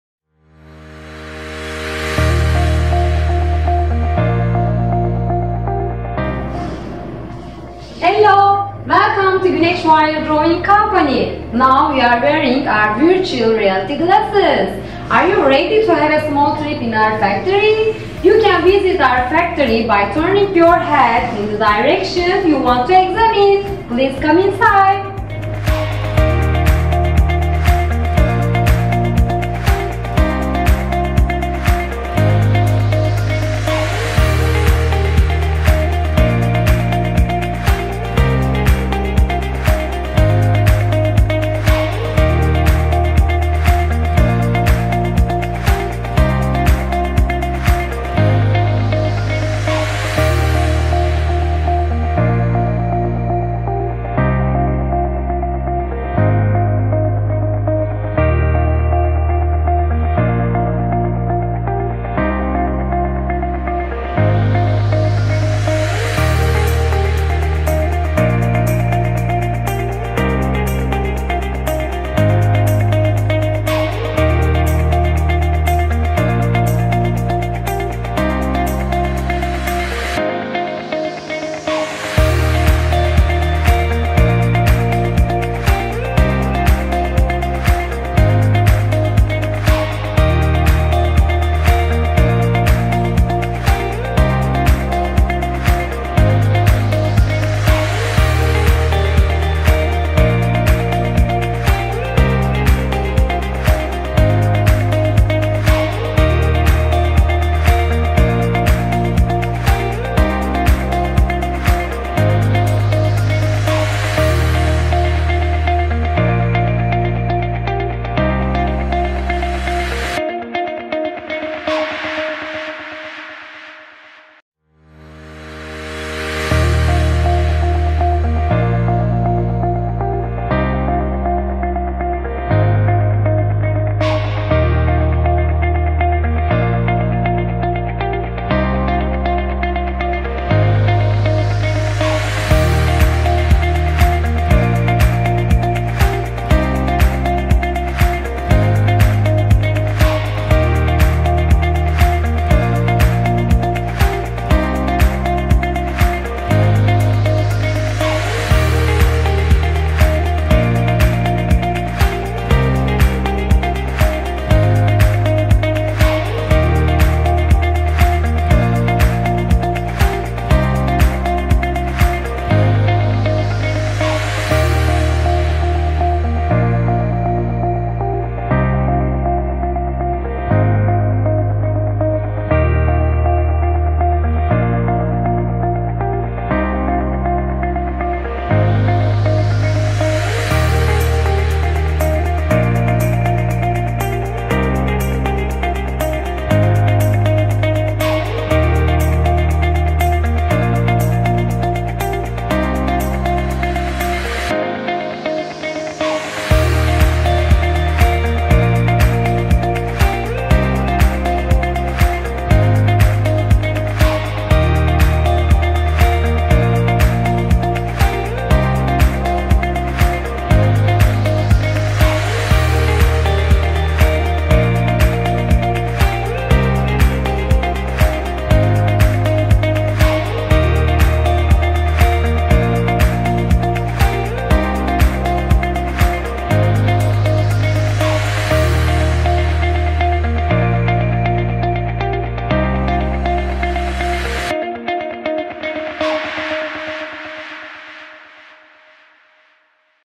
Güneş Tel Çekme - 360° Fabrika Tanıtımı
Güneş Tel Çekme için hazırlamış olduğumuz 360° fabrika tanıtım filmi.